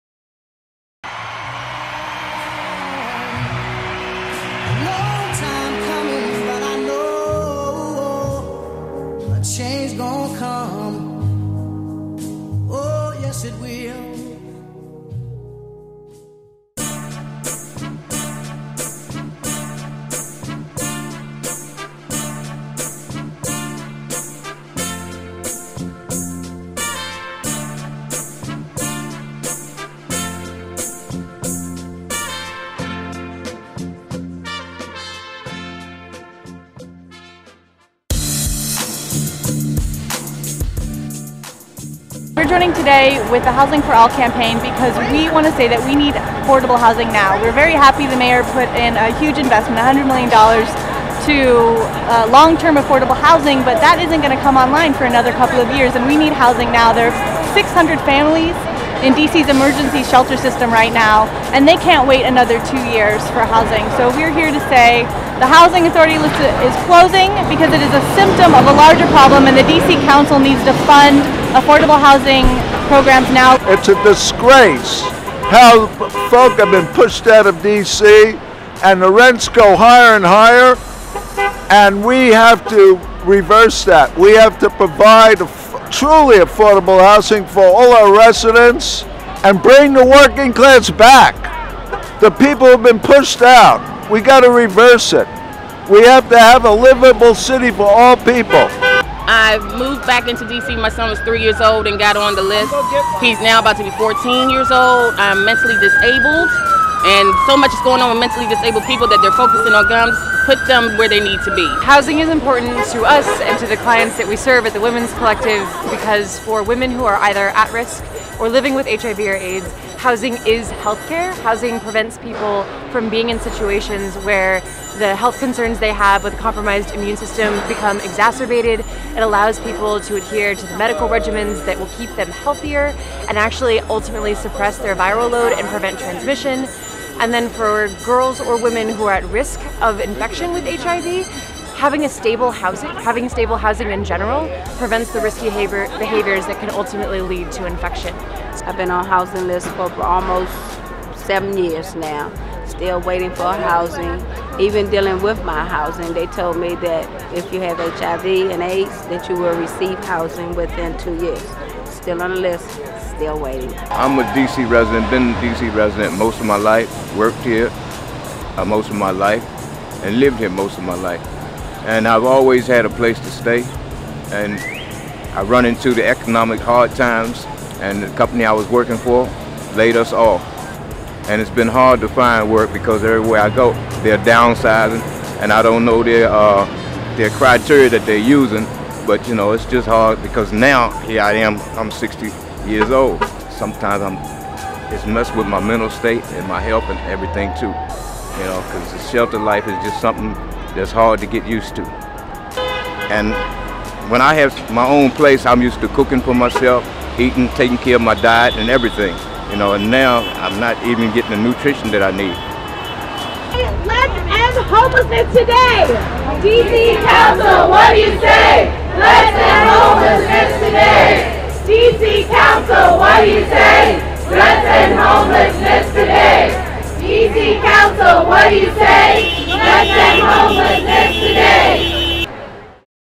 DC Fair Budget Coalition-DC Housing Protest
DC Housing Protest in front of the DC Housing Comission building in NW Washington, organized by the Fair Housing Coalition, to bring a demand to the need for afordable housing, especially for those that have been waiting for the longest time.